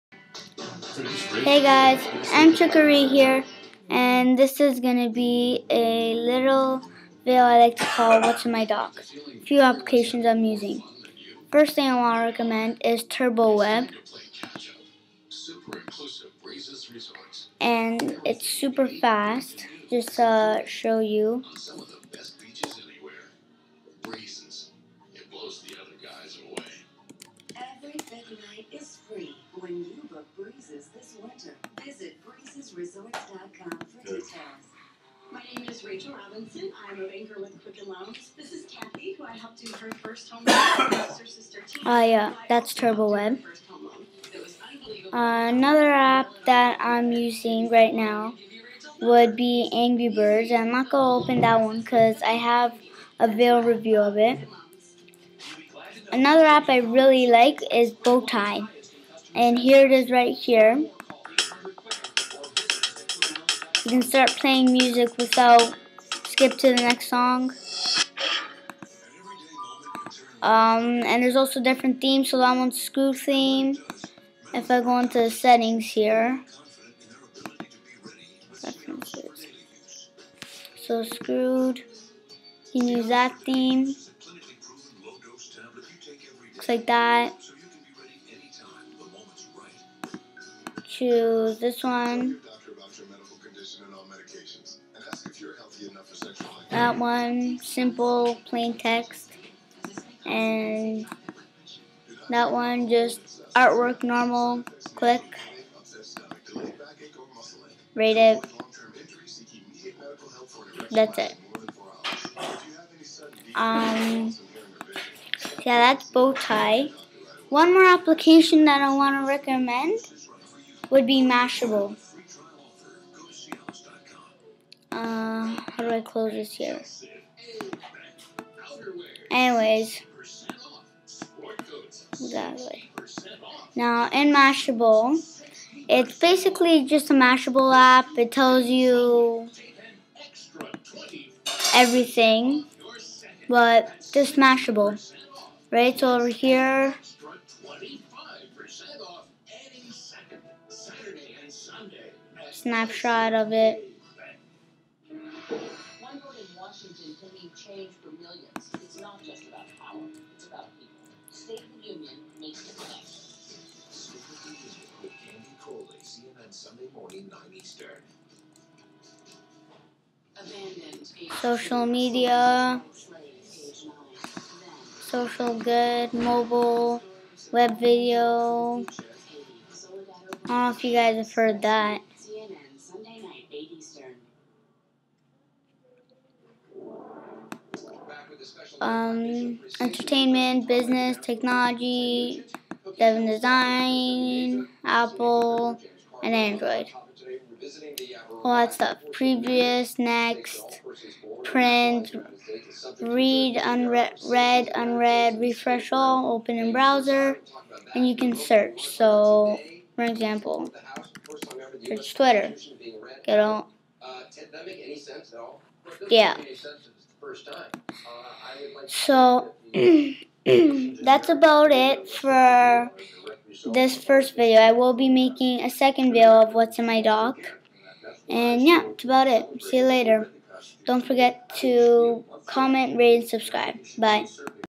whats in my← dock
these are a few apps for my mac from the app store that i reccomand